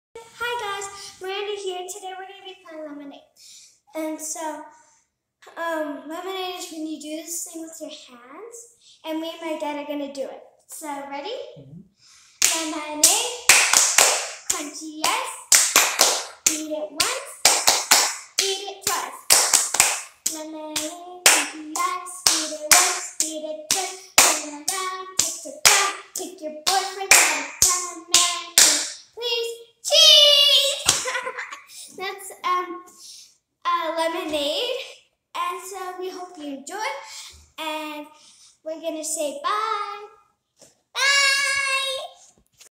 Let's play lemonade!
Miranda plays lemonade with her Dad.